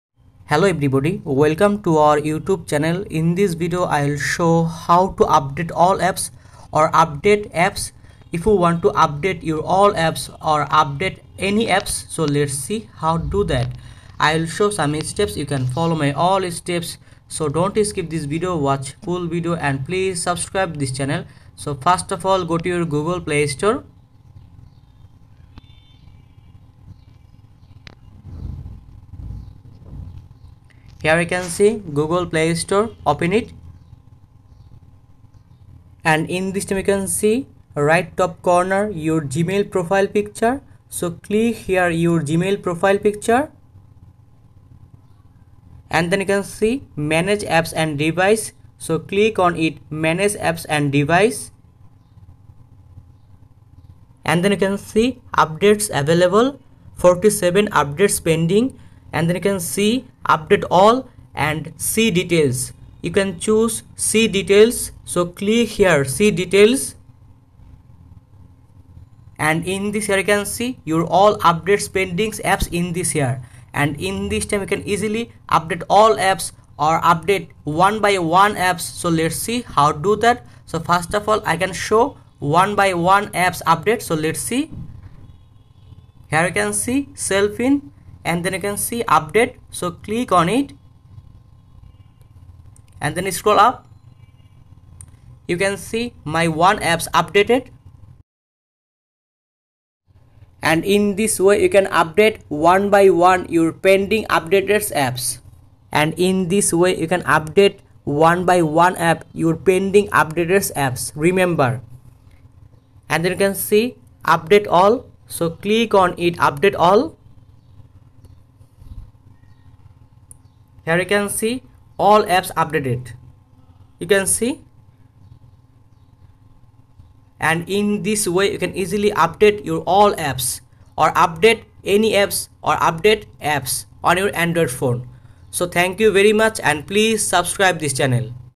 How To Update All Apps in Android Phone
In This Video I Will Show How To Update All Apps in Android Phone.

If You Want Update Your Phone Apps You Can Follow This Video All Steps & Then You Can Easily Update Your All Apps in Android.